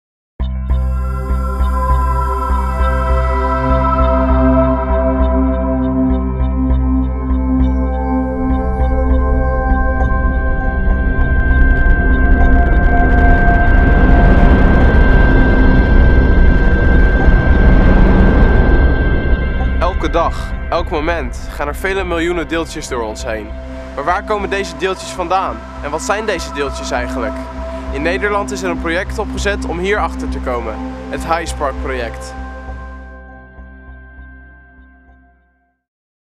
HiSparc Documentary Intro
The final intro for a documentary about the dutch HiSparc project. The audio still has to be fixed, but I can't really figure out what's the problem.